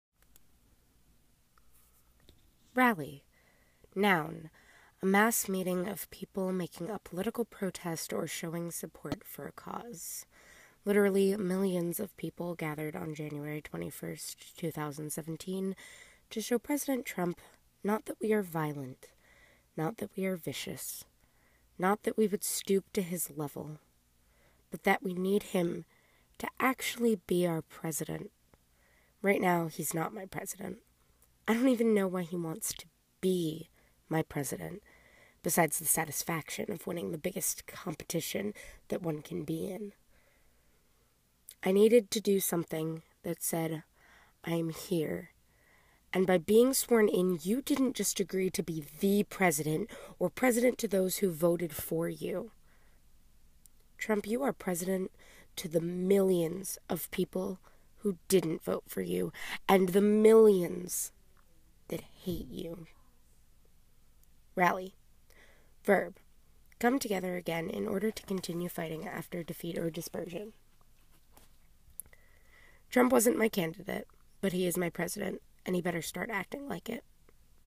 Why We March- Trump was Not My Candidate
A spoken word/thoughts from places about the women's rally in Lake Eola

No matter your party or feelings on president trump I hope you can enjoy this video and agree with the idea that our president should be representative of all americans :)